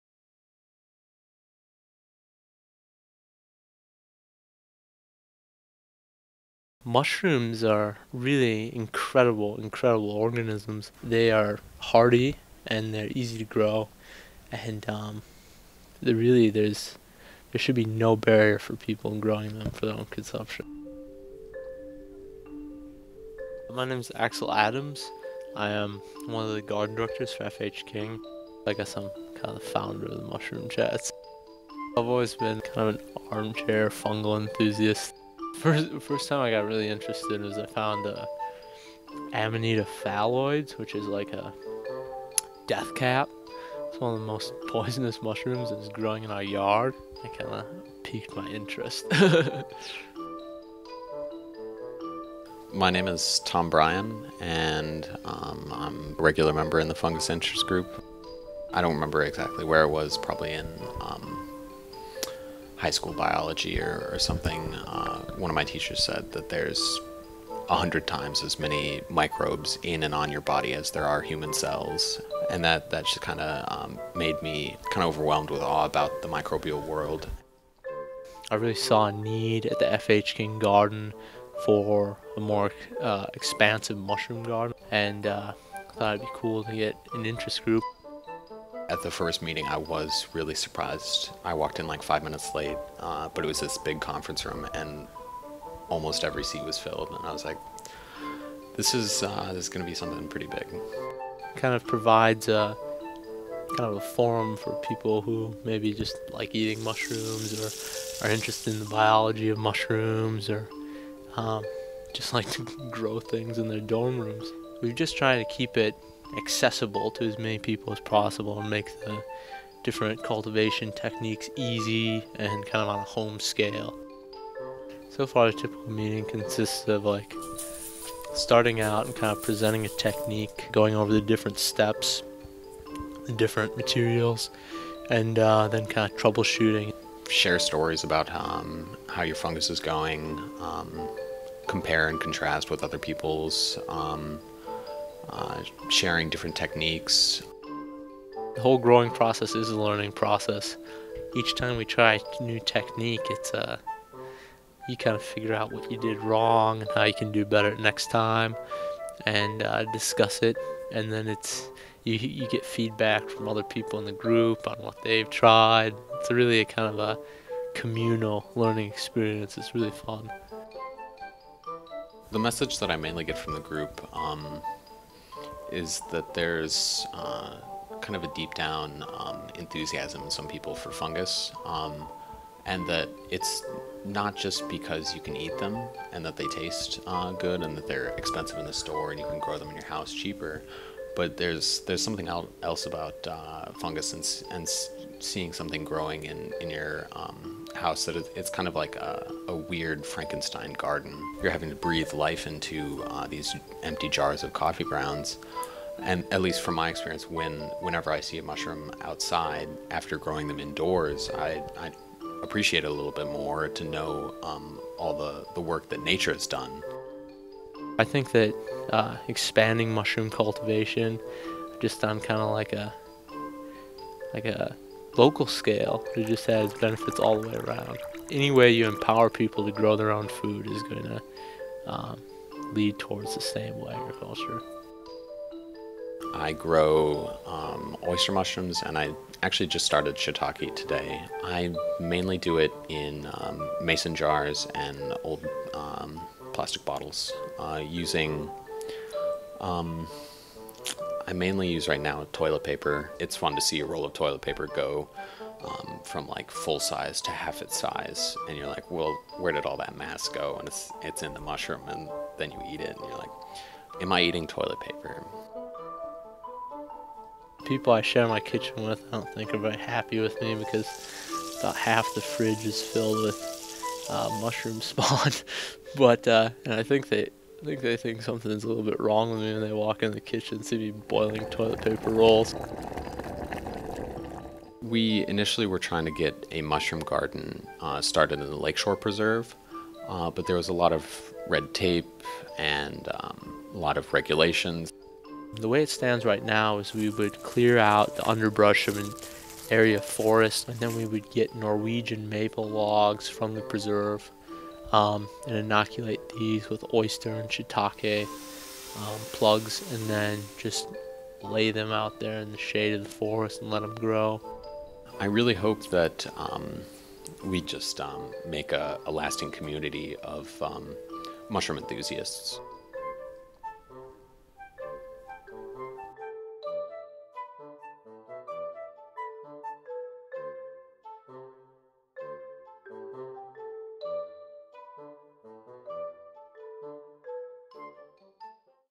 Growing Mushrooms (the legal kind)
Are you an "armchair mushroom enthusiast"? Do you shy away from fungus because it's too expensive at your local grocery store? Meet Axel and Tom, two students at the University of Wisconsin-Madison who are promoting a more sustainable way to get your daily intake of shiitakes and oyster mushrooms.

by Jasmine Badreddine and Rachel Schroeder